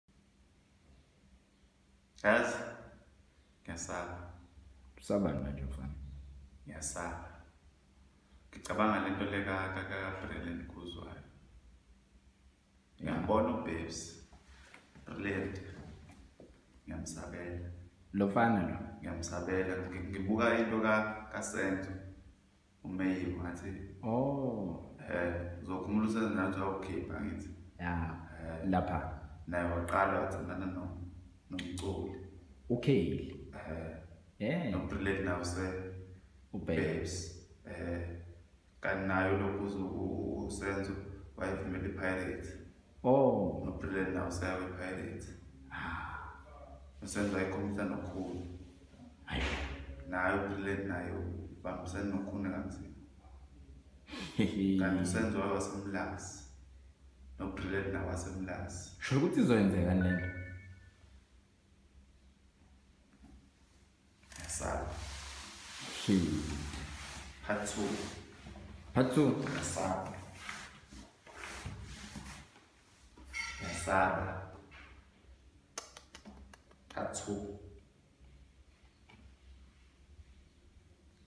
Babes wodumo and Brilliant Khuzwayo ~ Siyanda Maphumulo
It seems like Brilliant Khuzwayo has forgotten what happened to the late Senzo Meyiwa when he dated the musician (Kelly Khumalo) it’s clear musicians and soccer players should keep distance, otherwise we’ll lose more goalkeepers in South Africa.